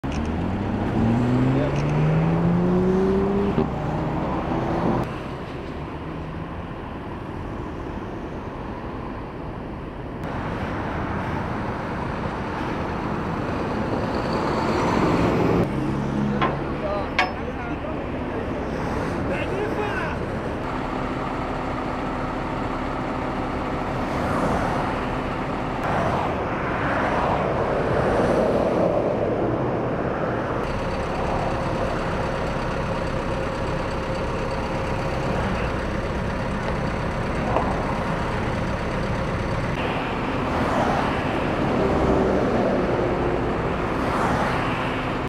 Lange files door kop-staart aanrijding De Wijk
De Wijk - Bij een kop-staart aanrijding op de A28 ter hoogte van De Wijk is een behoorlijke verkeerschaos ontstaan. Diverse auto's reden in de spits achter op elkaar. Waarbij zes personenauto's moesten afgesleept. Door het ongeval ontstonden lange files op de A28 tussen Hoogeveen en De Wijk. Rijkswaterstaat meld dat het verkeer over de vluchtstrook word omgeleid. Door het ongeval is 6km file ontstaan.